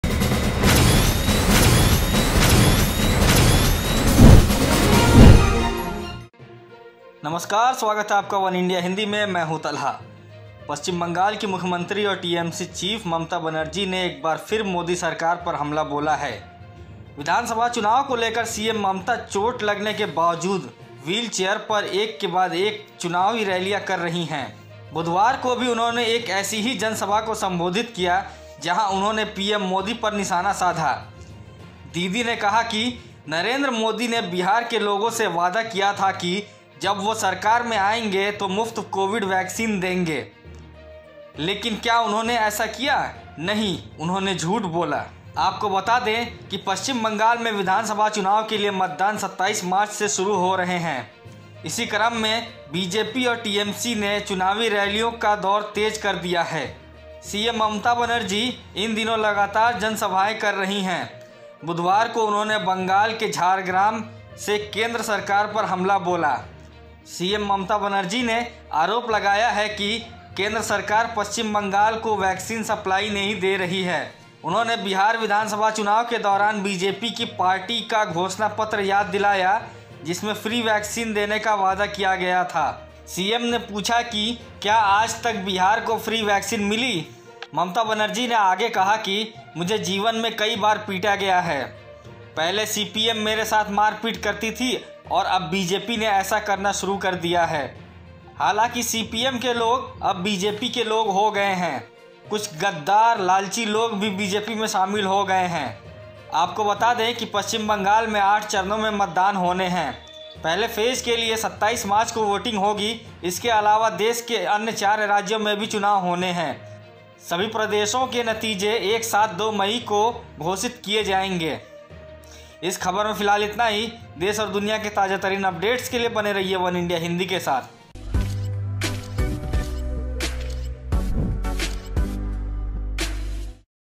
Bengal Election 2021: Mamata Banerjee ने PM Modi को बताया झूठा, पूछा ये सवाल | वनइंडिया हिंदी
Bengal Election 2021: Mamata Banerjee told PM Modi a liar, asked this question. West Bengal Chief Minister and TMC Chief Mamta Banerjee has once again attacked the Modi government. CM Mamata is holding election rallies one after the other in wheelchairs despite injuries.

पश्चिम बंगाल की मुख्यमंत्री और टीएमसी चीफ ममता बनर्जी ने एक बार फिर मोदी सरकार पर हमला बोला है. विधानसभा चुनाव को लेकर सीएम ममता चोट लगने के बावजूद व्हीलचेयर पर एक के बाद एक चुनावी रैलियां कर रही हैं.